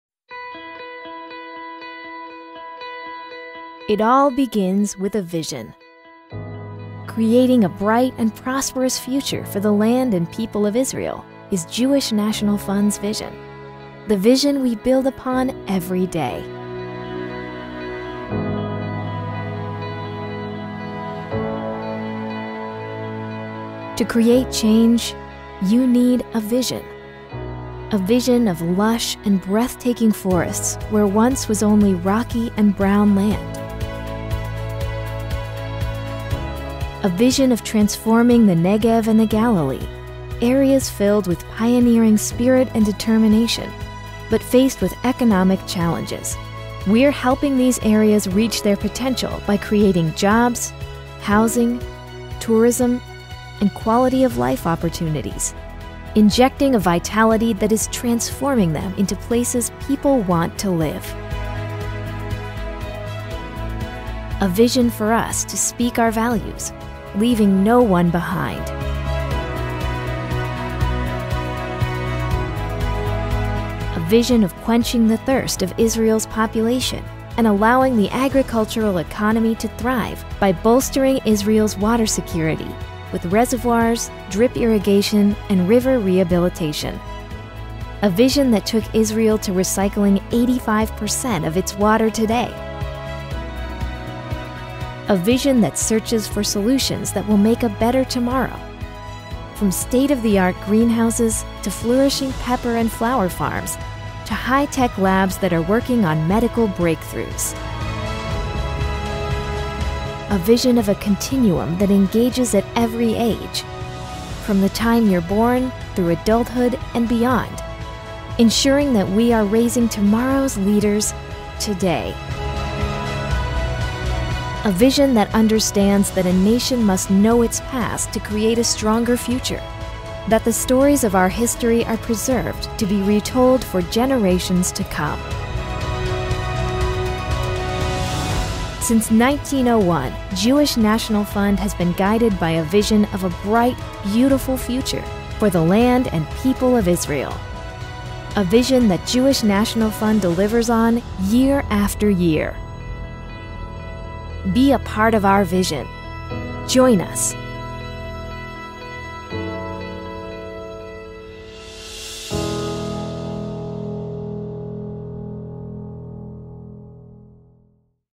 It Starts with a Vision: Updated
Since our founding in 1901, our single driving focus has been to ensure a strong, secure and prosperous Israel for the Jewish people everywhere. And our results speak for themselves.

Together with our partners, we re-established our ancestral Jewish homeland in modern Israel. We planted more than a quarter-billion trees in what was once a barren desert. And we built hundreds of vibrant, bustling, life-affirming communities throughout Israel - including the modern metropolis of Tel Aviv. But, while we’re proud of our success, we have little interest in looking backward.